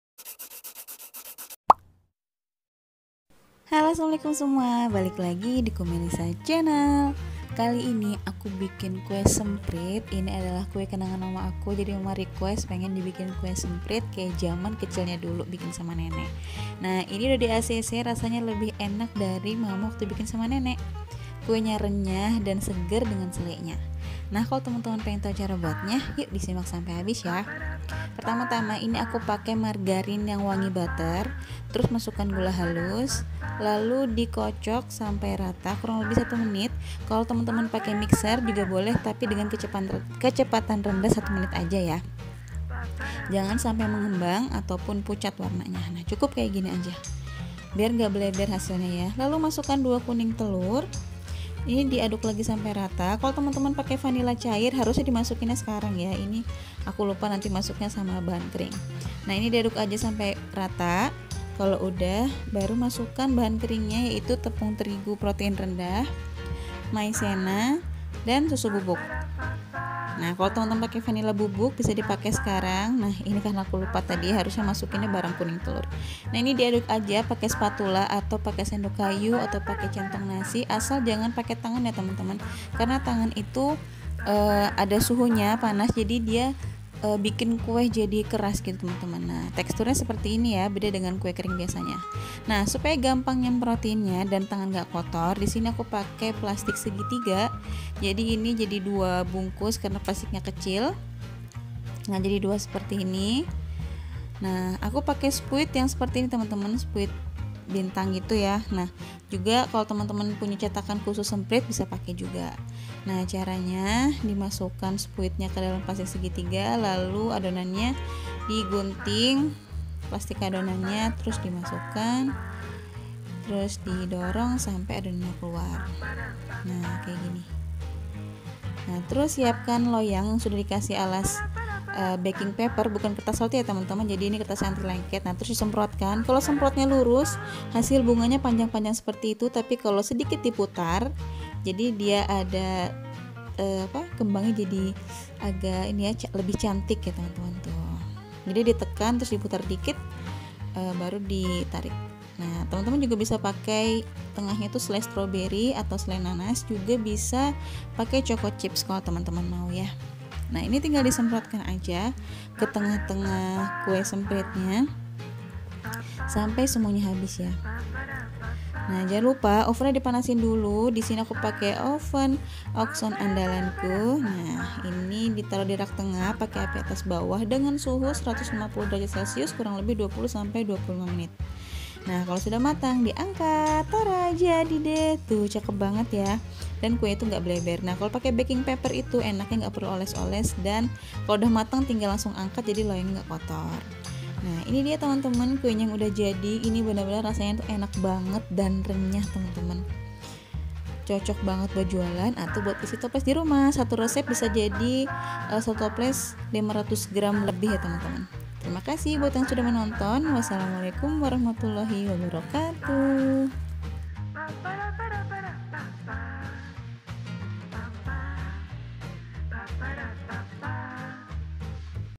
KUE SEMPRIT KENANGAN NENEK
Haii, assalamu'alaikum semua..
Niih request mamah pengeenn banget dibikinin kue semprit.. inget alm nenek katanya.. suka bikin kue semprit bareng dan mamah bagian manggang.. kenangan manis :')
Begitu kue ini jadi, malemnya dimakan..
Aq kaget dong, mamah makan kuenya sambil nangis 😂 katanya inget neneeek, tapi kue ini lebih enak dr buatan nenek 😁 alhamdulillah
Yang pengen bikin yuukk, penuhin toples dirumah.. jangan abis duluan tapi 😄

Kue Semprit
Hasilnya 500 gr

Bahan :
200gr margarin aroma butter
100 gr gula halus
2 bh kuning telur
1 sdt vanilla esens/bubuk
250 gr tepung terigu protein rendah
50 gr maizena
10 gr susu bubuk
Hiasan/toping :
Selai nanas/stoberry/choco chips
Kalo temen² udh recook, boleh tag/mention instagram aku yaaa.. insyaa Allah aq repost di Story.. terimikichii 💕

Wassalamu'alaikum warohmatullahi wabarokatuh